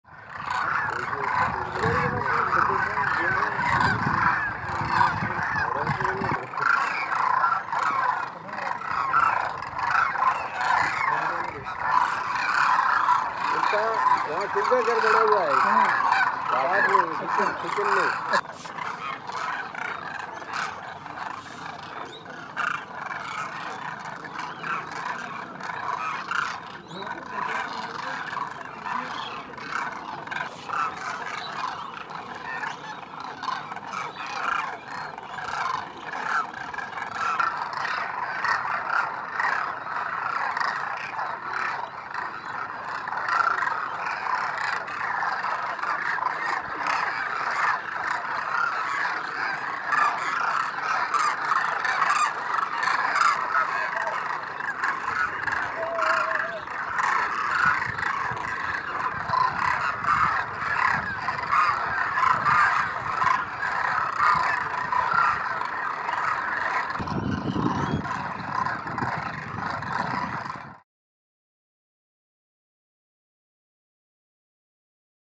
Thousands of migratory Demoiselle Cranes in Rajasthan
Demoiselle Cranes in massive numbers call away as they sit near a water hole in Rajasthan. They usually migrate to the northern Indian regions in winter. The cranes are preening their plumage.
In a village called Khinchan in Rajasthan, the cranes are welcomed by the locals with sackfulls of grains. The villagers feed them and they are treated like royalty.